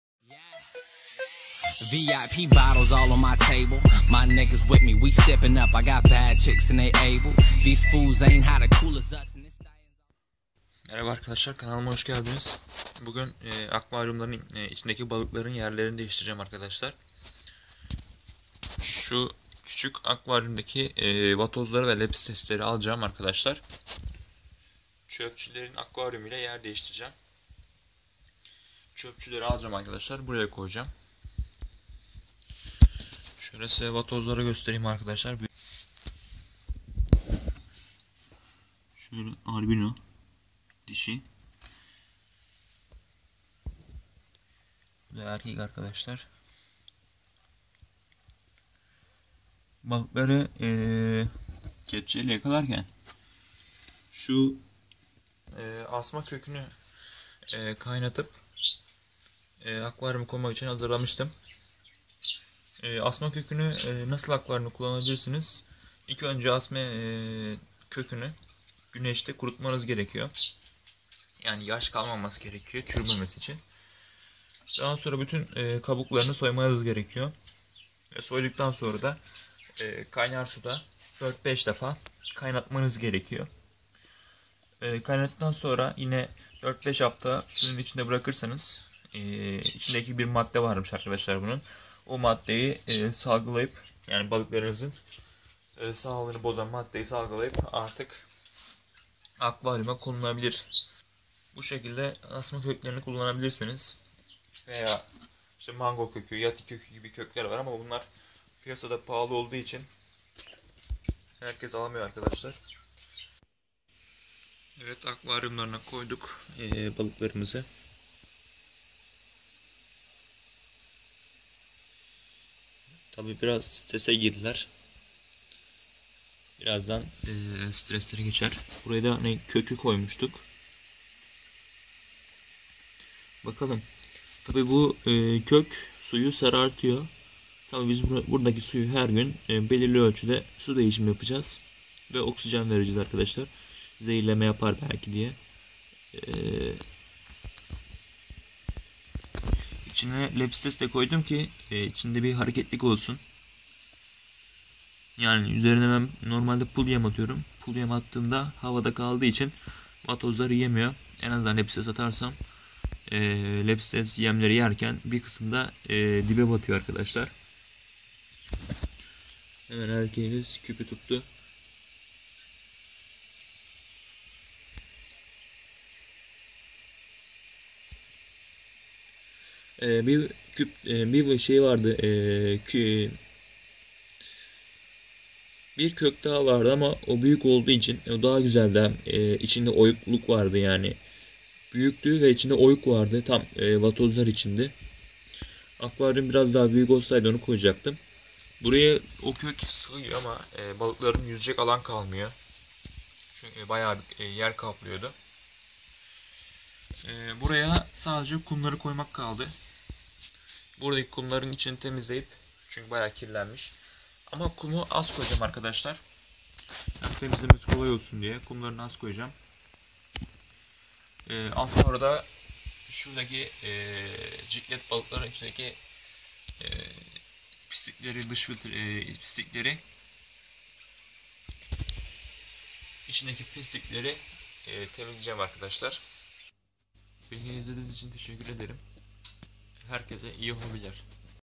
AKVARYUMA ASMA KÖKÜ KOYMA-AKVARYUMDAKİ BALIKLARIN YERLERİ DEĞİŞTİ 07.01.2018
ARKADAŞLAR VİDEO BEĞENDİĞSENİZ LİKE ATMAYI UNUTMAYIN